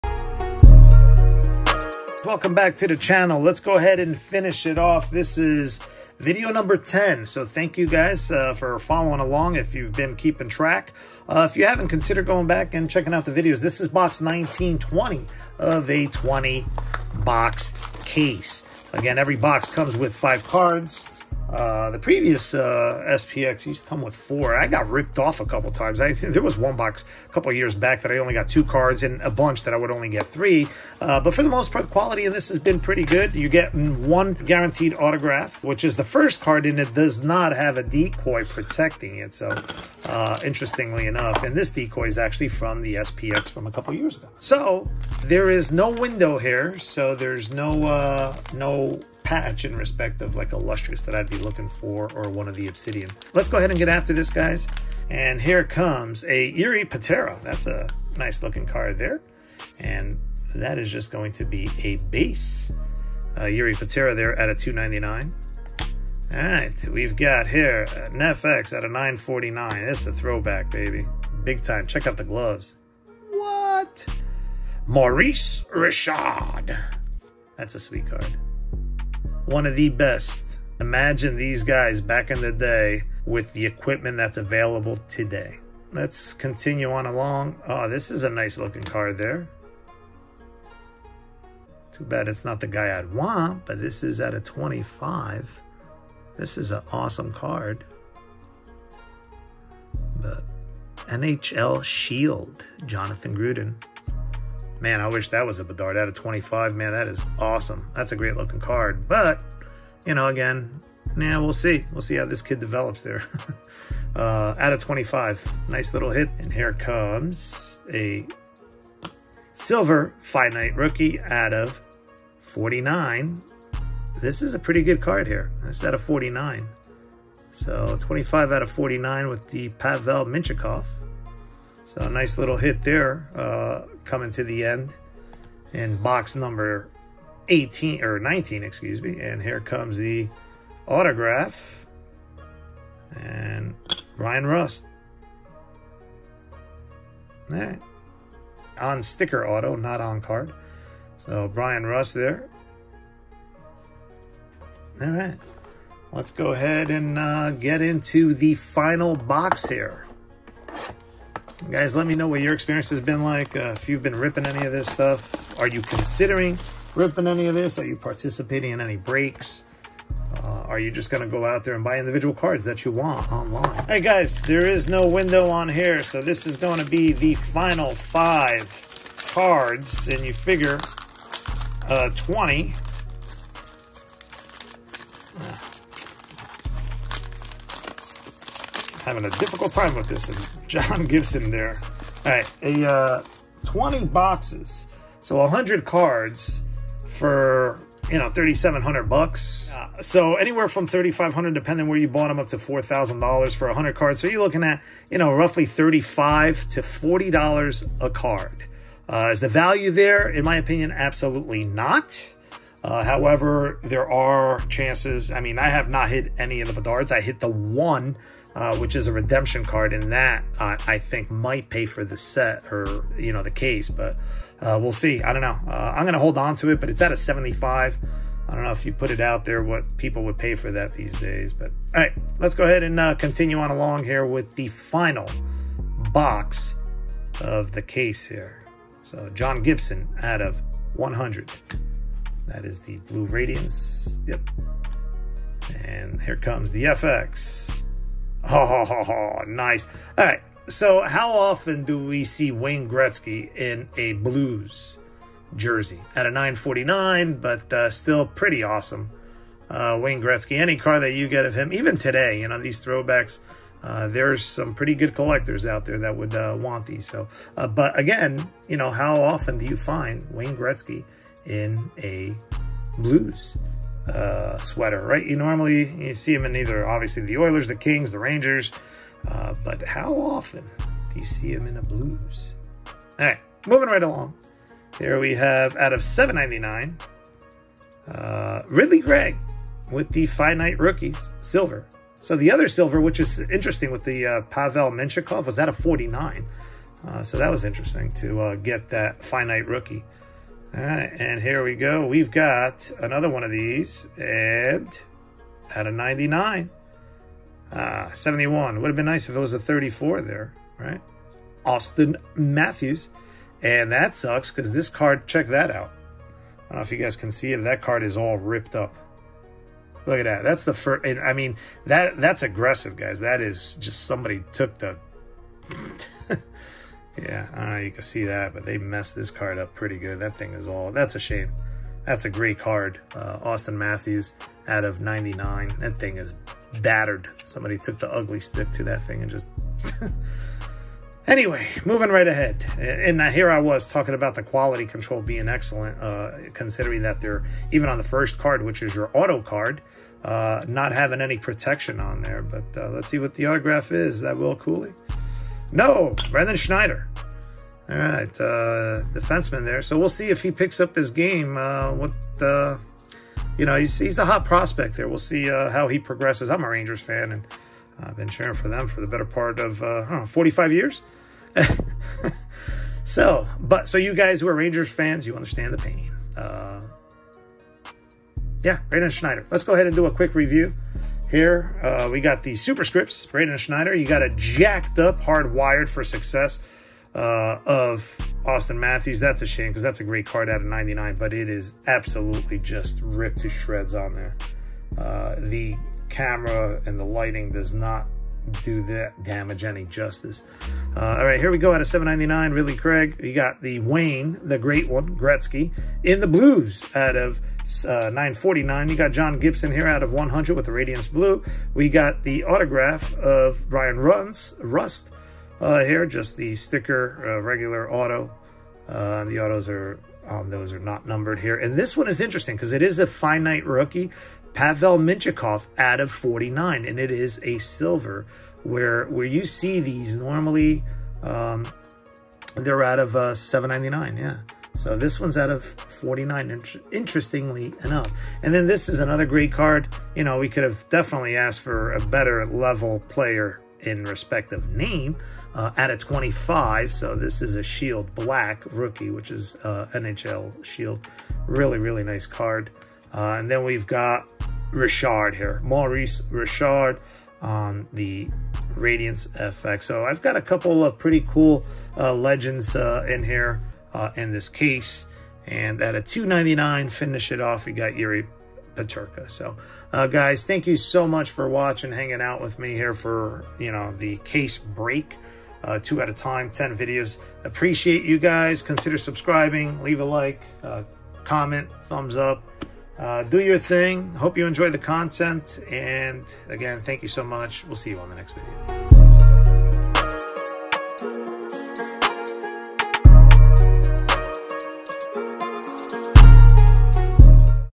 LAST BOX OF 2023-2024 SPx HOCKEY BOX UNBOXING !! Episode 86
I will be giving away 4 Blaster boxes  of 2023-24  Basketball, Baseball, Football and Hockey to 4 lucky viewers.  I will follow up with further instructions on how to participate when we hit the 100 Subs benchmark.